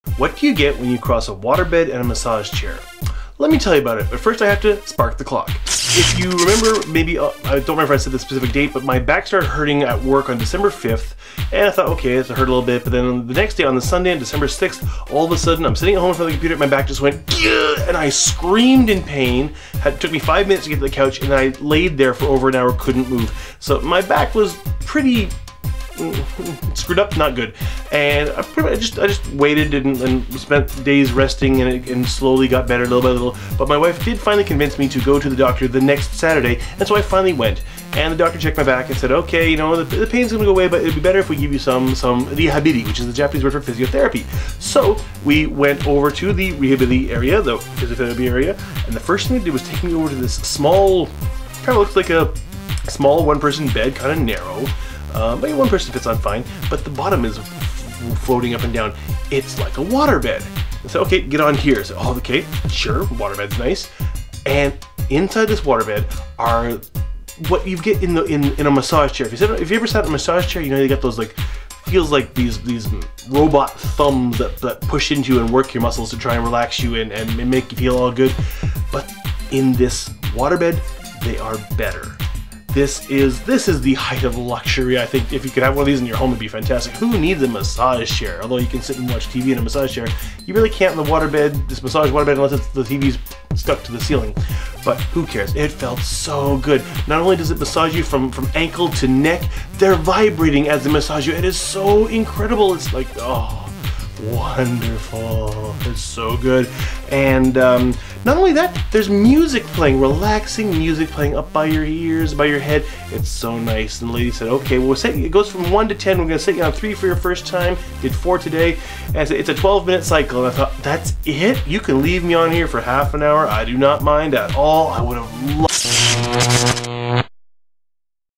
Japanese Massage Waterbed
I went to a clinic for my back, and visited again today, and I have fallen in love with one peice of equipment they have! I've never seen one before! It's what you get when you cross a waterbed with a massage chair. A massage waterbed! I WANT ONE! but will never buy one of course! :)

Vlogmas 2015 Day 21

If you have any suggestions for topics, or questions you'd like me to answer in one of these videos, please leave them in the comments!